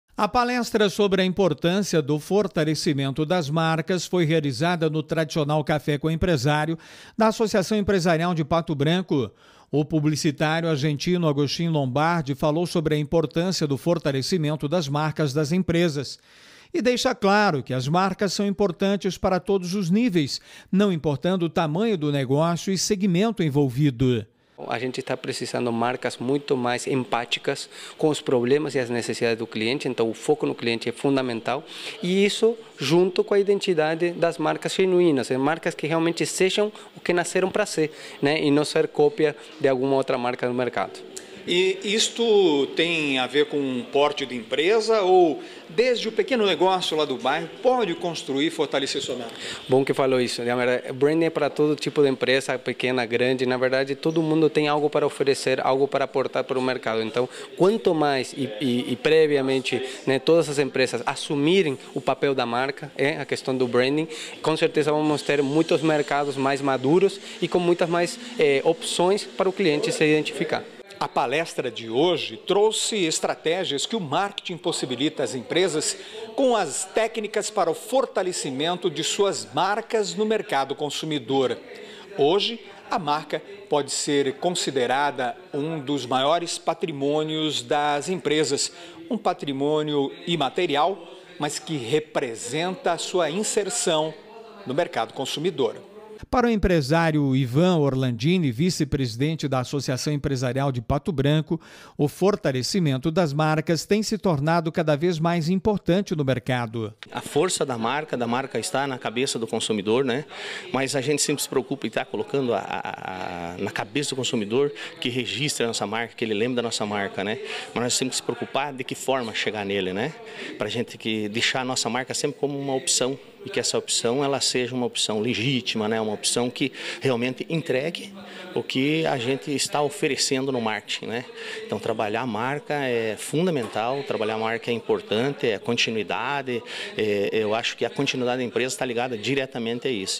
Palestra destaca importância de fortalecer marca
Eventos: Em Pato Branco, palestra tratou da importância do fortalecimento das marcas das empresas.